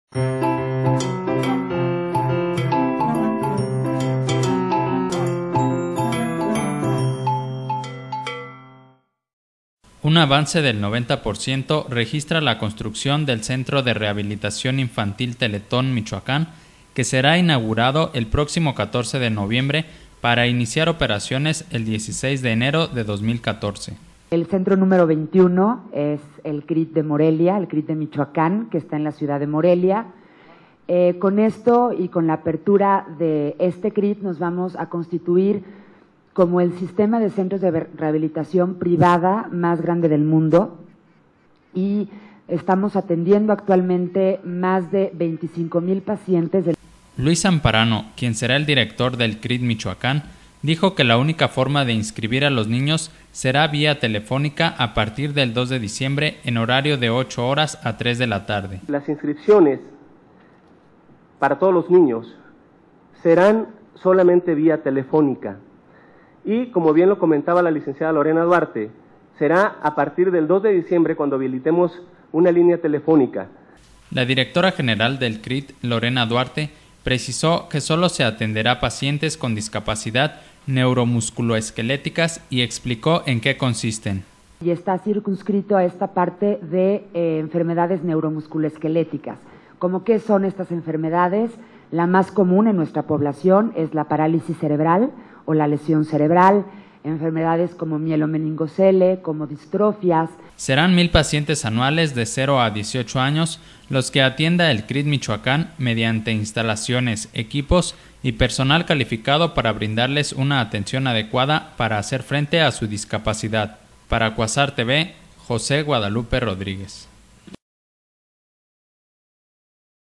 Inaugurarán CRIT Michoacán el 14 de noviembre
Un avance del 90 por ciento registra la construcción del Centro de Rehabilitación Infantil Teletón, Michoacán que será inaugurado el próximo 14 de noviembre para iniciar operaciones el 16 de enero del 2014...